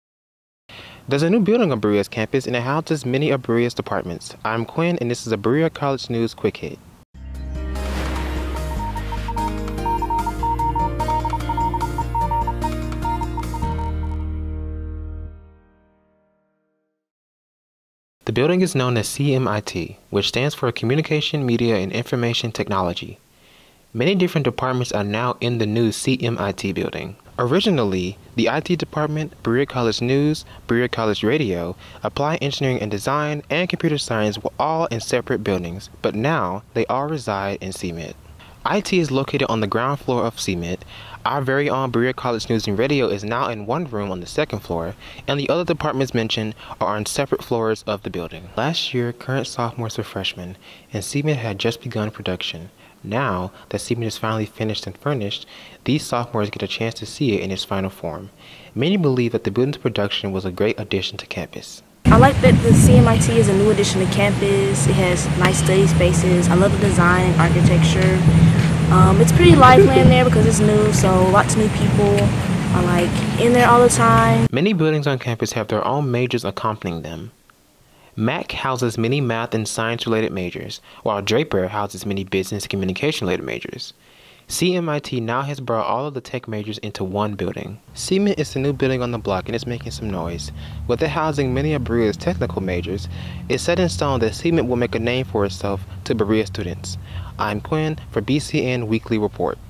CMIT Construction Completed
The recently constructed CMIT building is BCNR's new home! In this BCN Quick Hit, Quinn explores the building and its opportunities. Keep an eye out for our Open House event this fall! 🍂🏫🧡